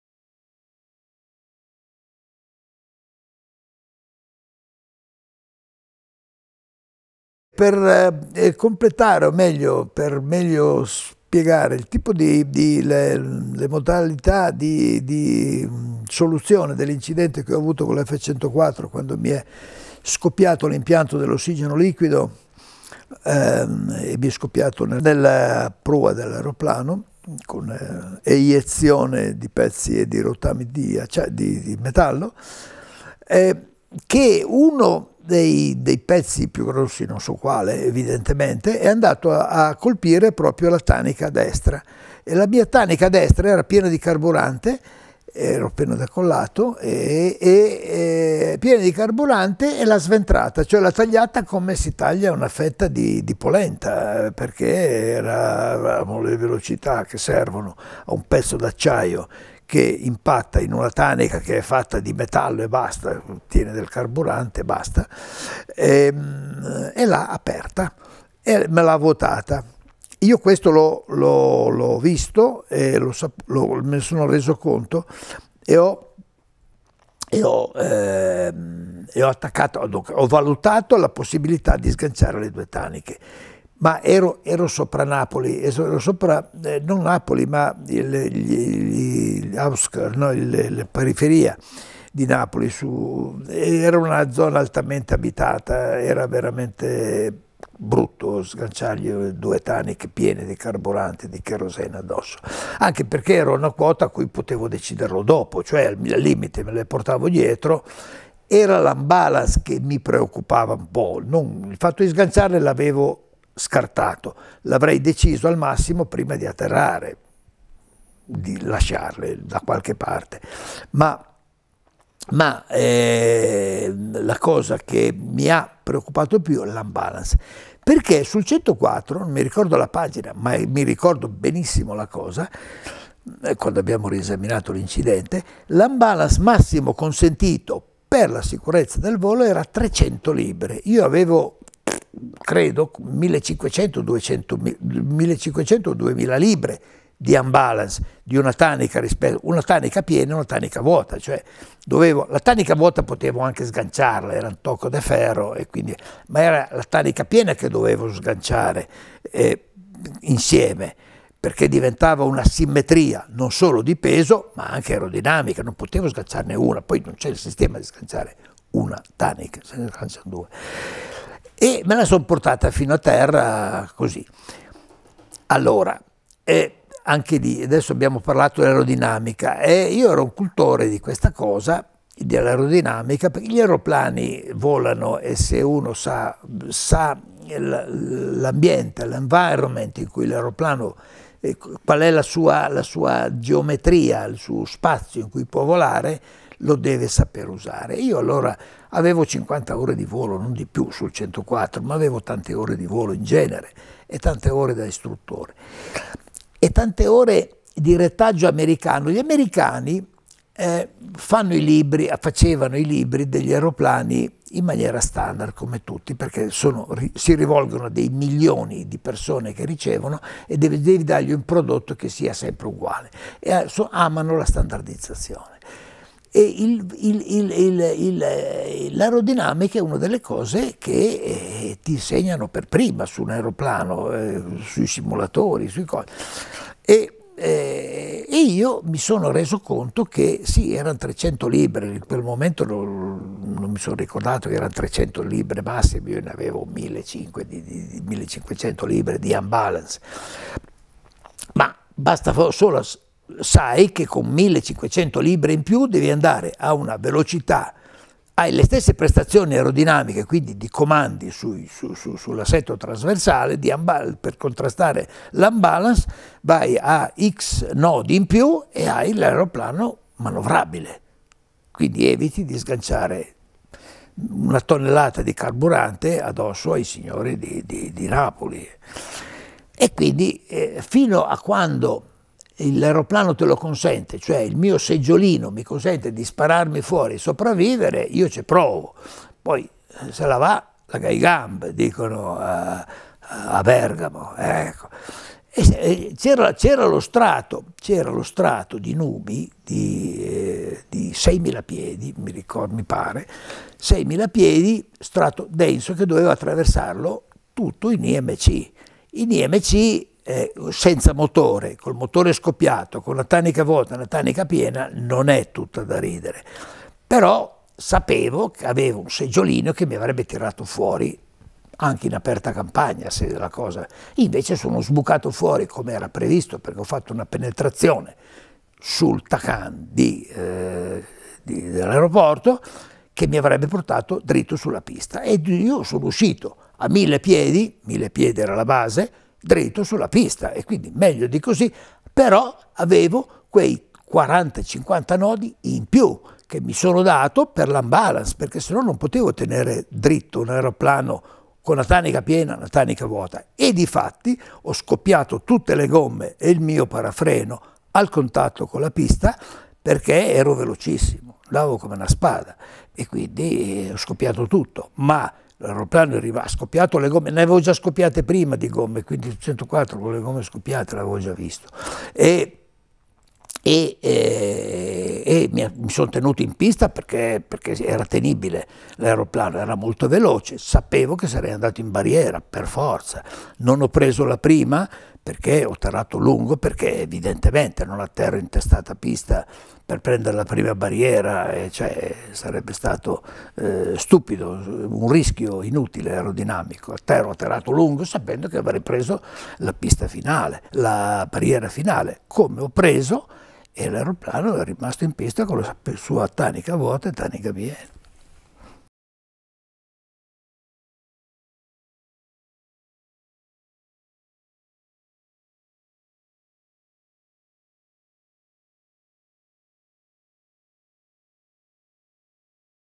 Atterraggio TF104 G motore spento TANICA DESTRA DANNEGGIATA E SBILANCIAMENTO DELL'AEREO - ELIA (7B)
Atterraggio di emergenza a motore spento con
TF 104 G - Gen. B. A. Gian Luigi Elia

VIDEO AGGIUNTIVO
INERENTE LA TANICA DESTRA DANNEGGIATA
E SVUOTATA CON CONSEGUENTE SBILANCIAMENTO DELL'AEROPLANO

video Claudio Costa  Lorenzo Ruffini
si ringrazia Riccardo Lalla